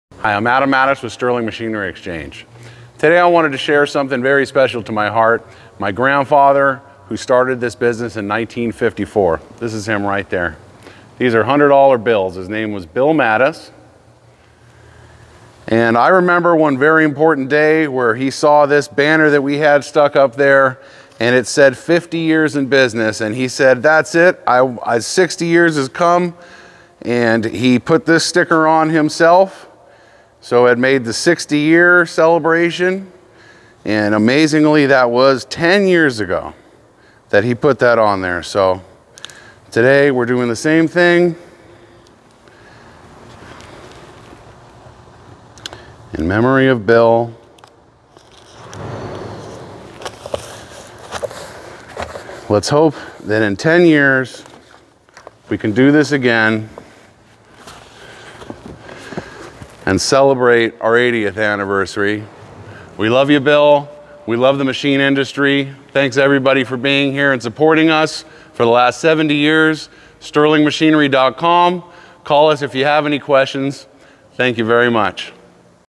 We did it! Sterling Machinery Exchange is 70 years old and still going strong.
We did it! Sterling Machinery Exchange is 70 years old and still going strong. It has been an absolute pleasure serving the medal working industry and Machine Tool industry Since 1954.

Join us while we update our Anniversary Banner!

We can't wait to show you what we have in store for the future. Please let us know how we can help you buy, sell, or trade Machinery today.